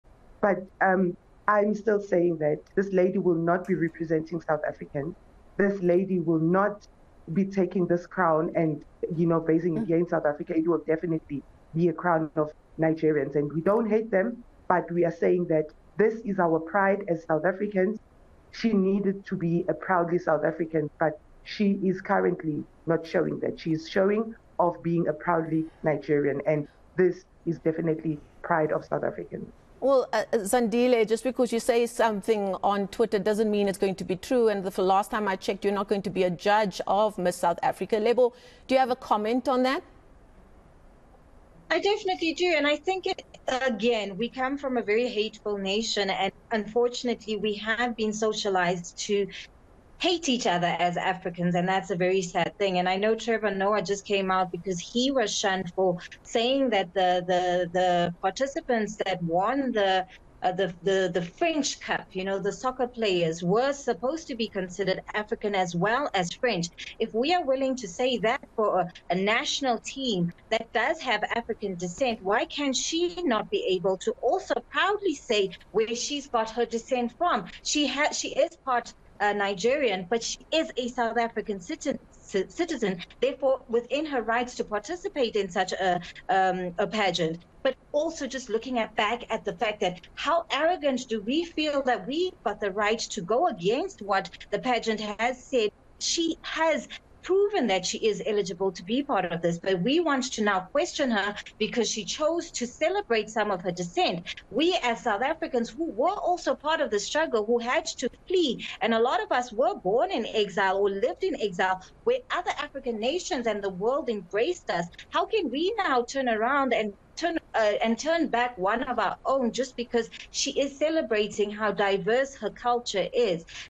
Miss SA 2024 | Pageant says Adetshina meets all requirements
The country remains divided over the citizenship of Miss SA 2024 finalist, Chidimma Adetshina. The 23-year-old believed to have a Nigerian father and South African mother has been facing backlash.